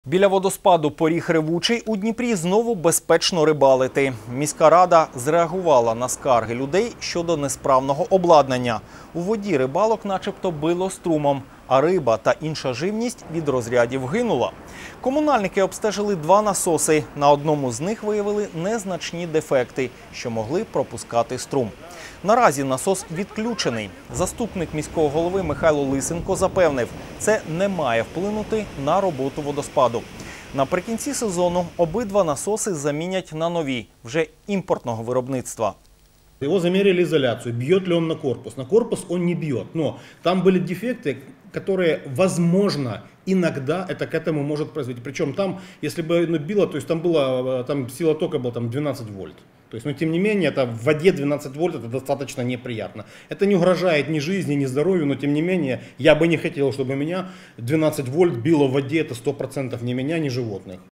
Водоспад “Поріг ревучий” більше не б'ється струмом
Біля водоспаду “Поріг ревучий” у Дніпрі знову безпечно рибалити. Міська рада зреагувала на скарги людей щодо несправного обладнання — у воді рибалок начебто било струмом, а риба та інша живність від розрядів гинула. Комунальники обстежили два насоси — на одному з них виявили незначні дефекти, що могли пропускати струм. Наразі насос відключений. Заступник міського голови Михайло Лисенко запевнив: це не має вплинути на роботу водоспаду. Наприкінці сезону обидва насоси замінять на нові, вже імпортного виробництва.